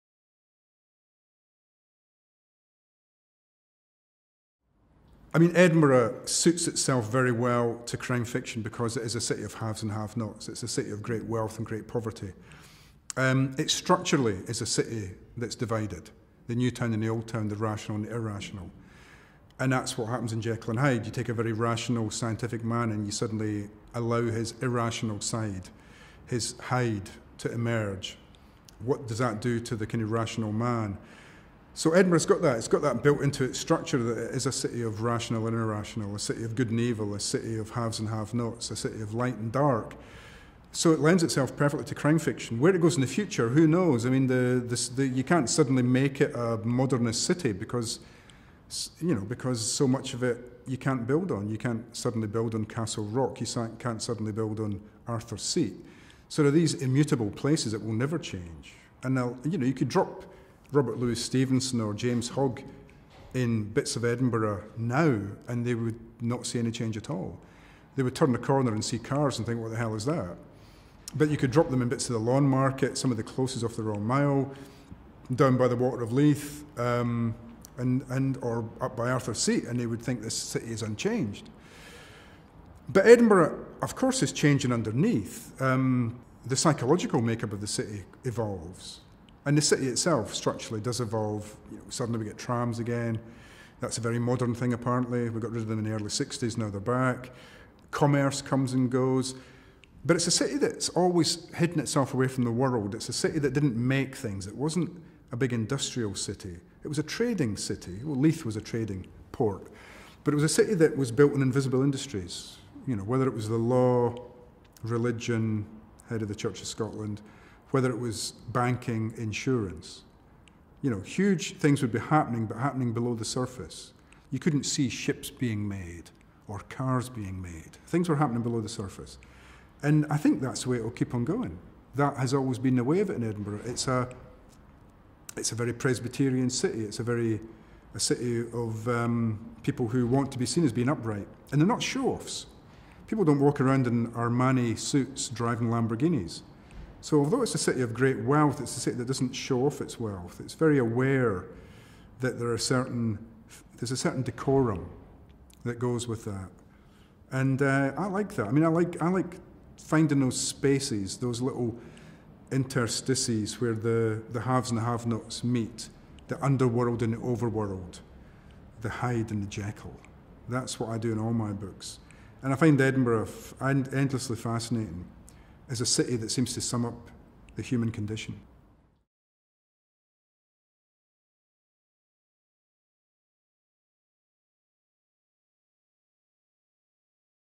Ian Rankin: The City of Edinburgh
Continuing Heriot-Watt's interview with Ian Rankin, he discusses Edinburgh - it's future, its past and its essence.

This video forms part of our interview with Ian about Dame Muriel Spark, Heriot-Watt graduate and author of, amongst other works, the Prime of Miss Jean Brodie. To find out more, download the Heriot-Watt University alumni magazine